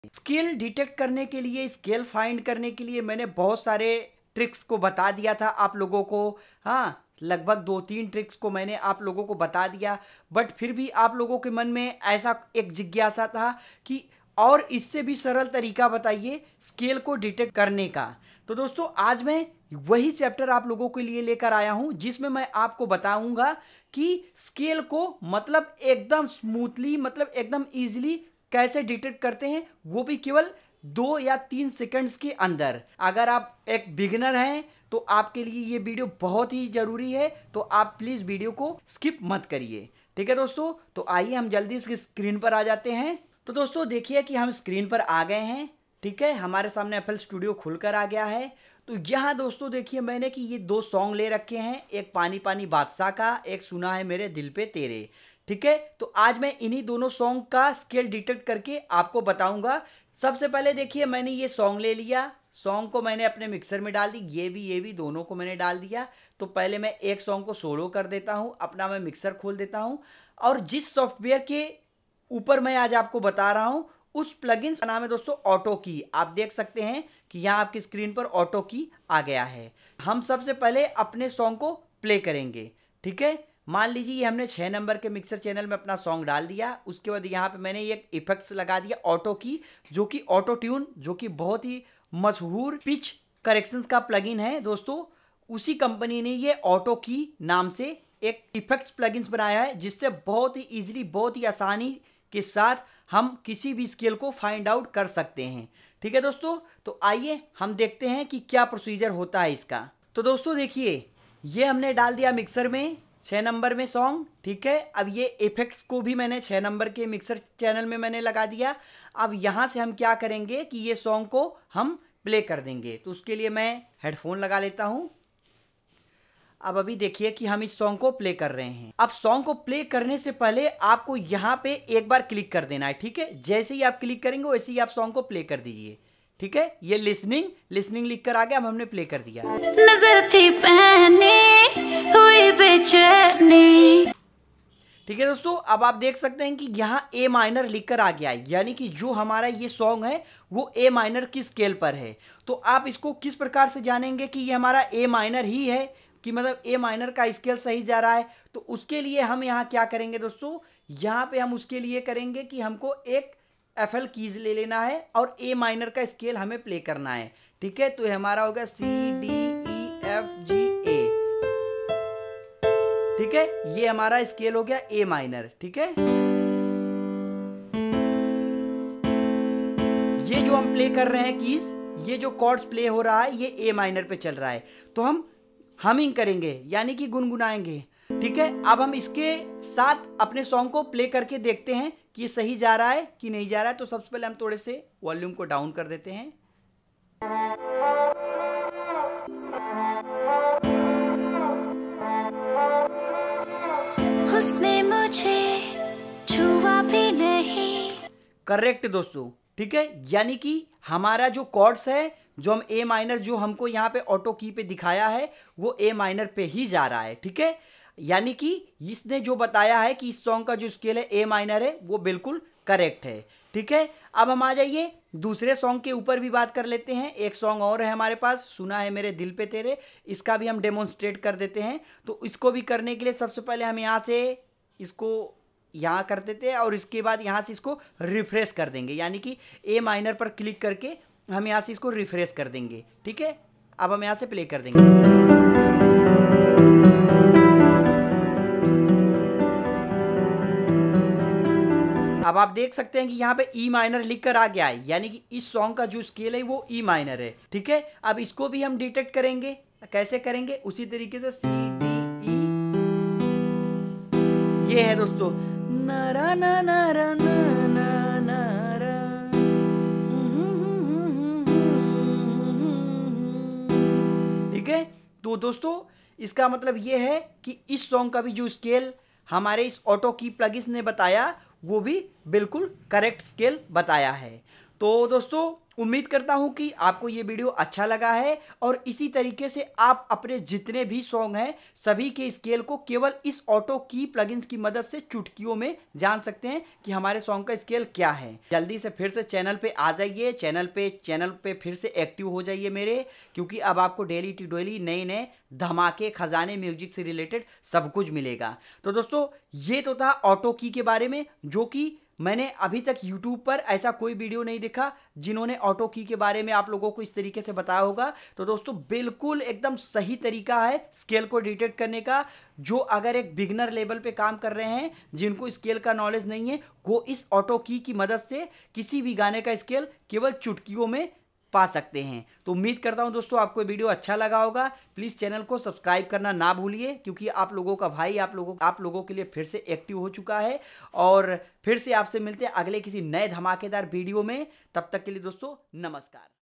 Scale Nikalne Ka Easy Formula | How to detect Scale in FL Studio 21.1
How to detect Scale in FL Studio 21.1 | Scale Nikalne Ka Easy Formula

About - In this video I'll explain how to detect scale in easy way